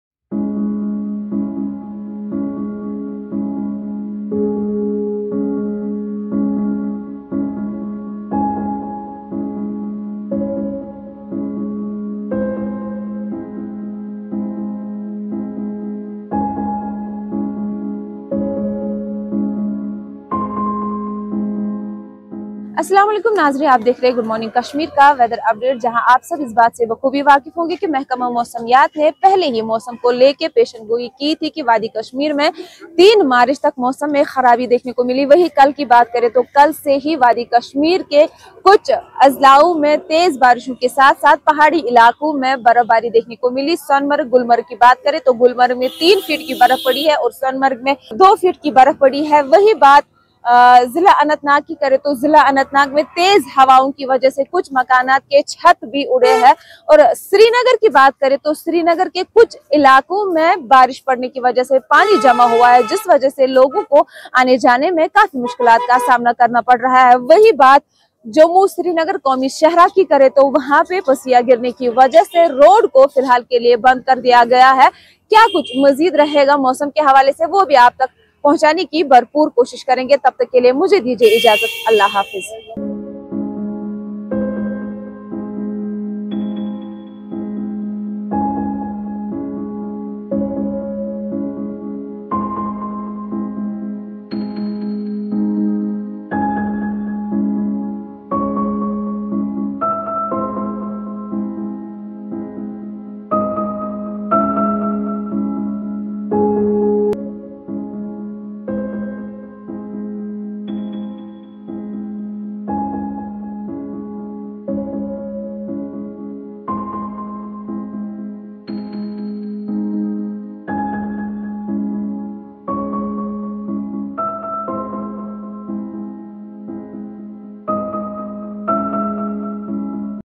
Rains lash the plains, while higher reaches are blanketed in heavy snowfall
Camera & Editing: Ashiq Naik